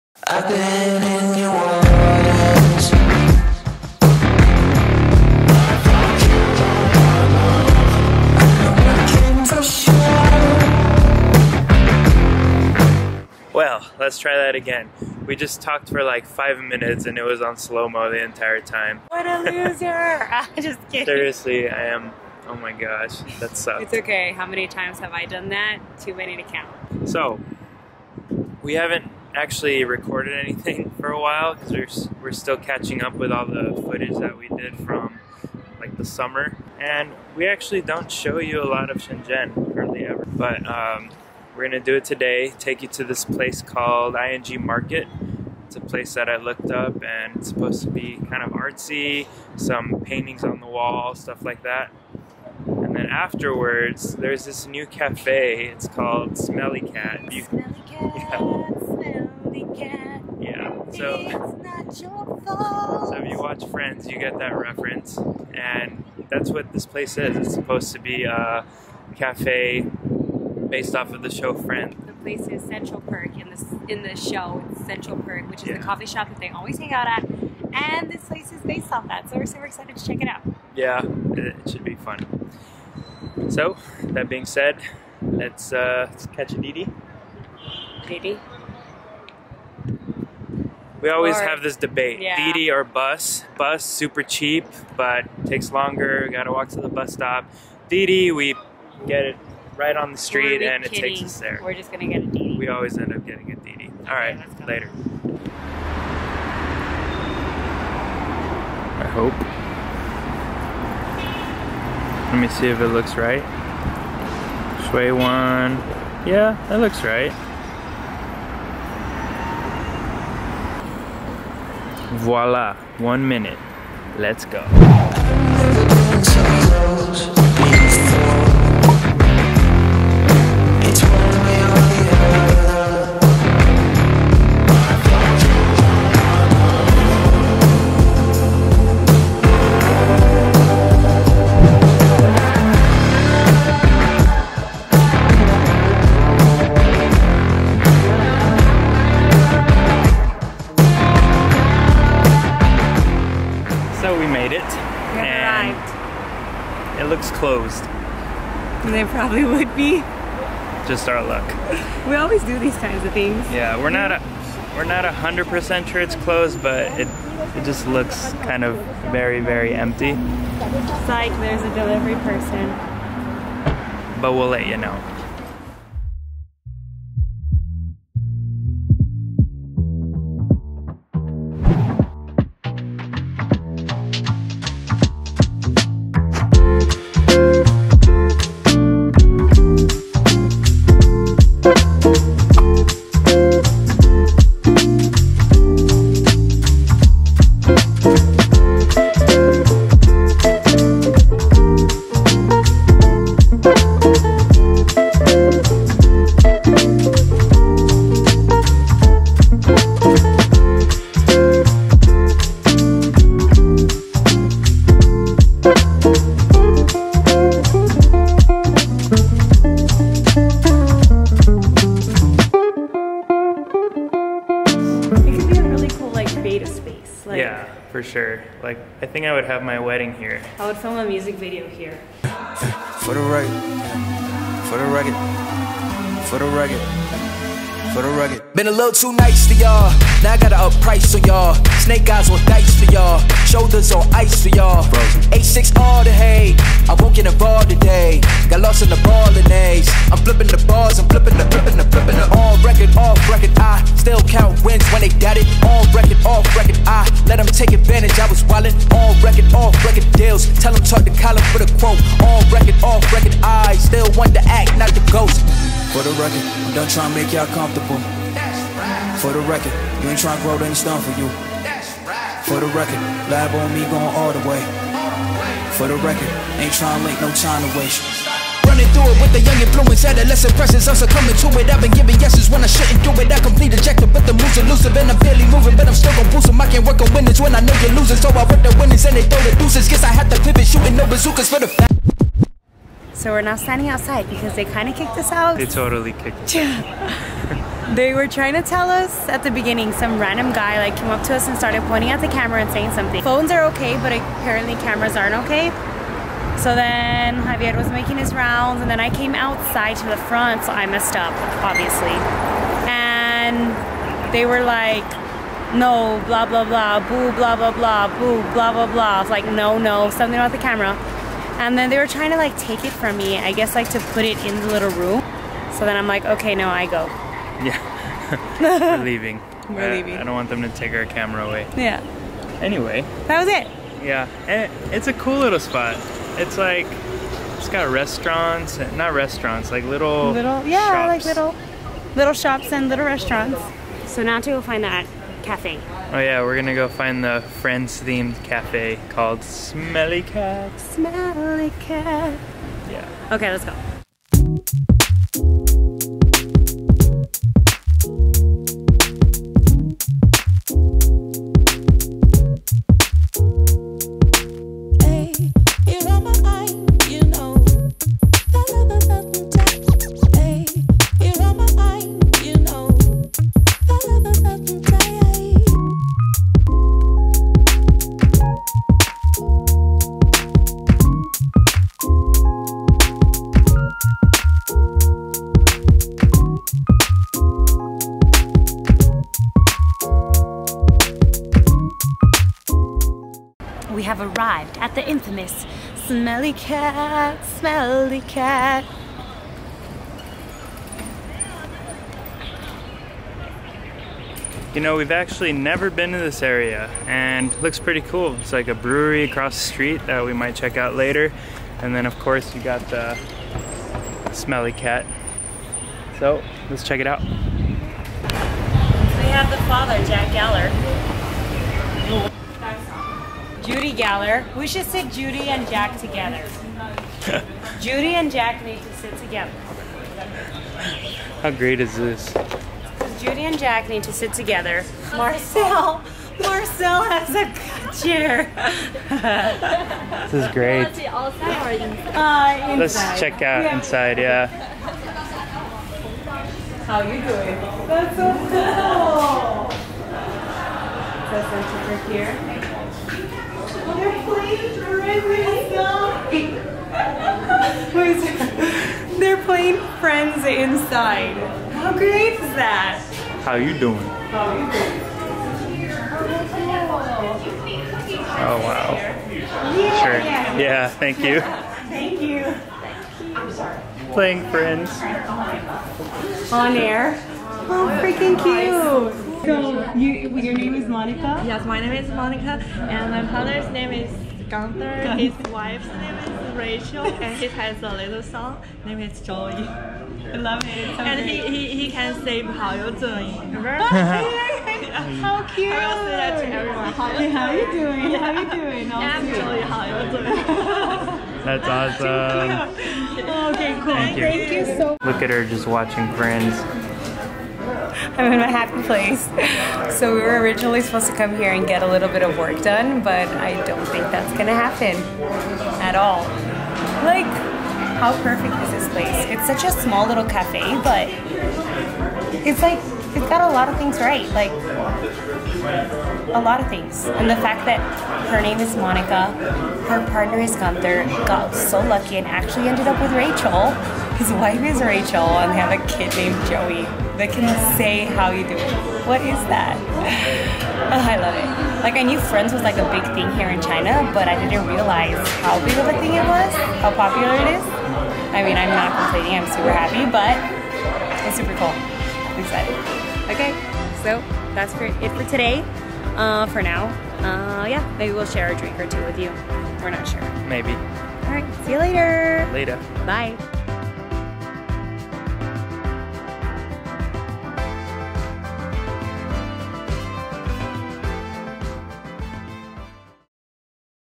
Shenzhen Vlog 1 | ING Market + Smelly Cat Cafe
Take a look at part 1 of the Shenzhen series. We don't share enough about Shenzhen but we're gonna be better about it.

Fun last minute video based on a day in Shenzhen, China. We take you on a tour of a local art venue and a new cafe based on the TV show Friends. Yes, it is as great as it sounds!

Smelly Cat Cafe
1F,Block B,Technolongy Buliding2,Gongye 6th Road,N
南海大道1057号科技大厦二期B座1楼
Technology Park 518000
Shekou

If you need to access socials in China you'll need a VPN! We recommend ExpressVPN

Tunes - Epidemicsounds.com